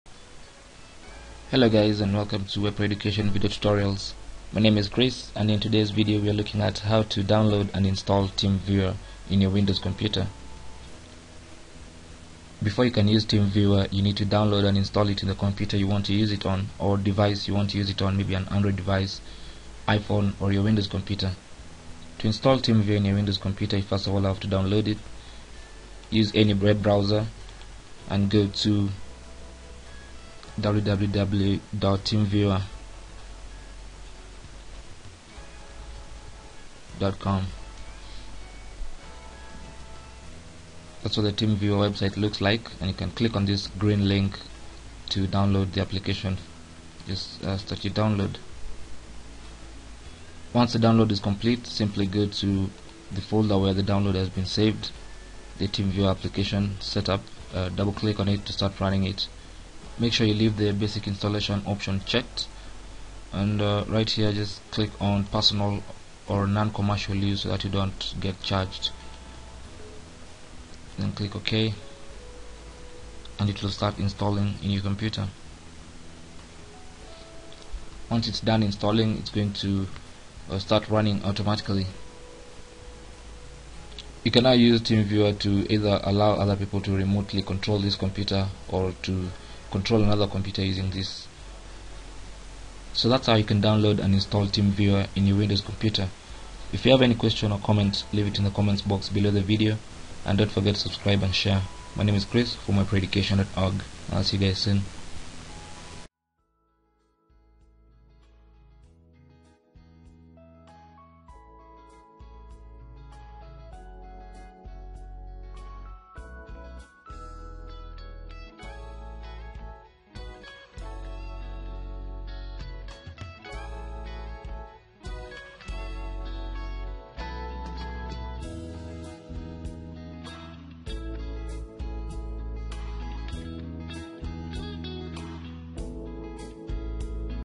How To Download and Install Teamviewer on Windows
Before you can use teamviewer,  you need to download and install it in the device or computer you want to use it on.

Click on the download link to save the application to your computer.
Double click on the application to start the install.
Follow the onscreen instructions to install the application.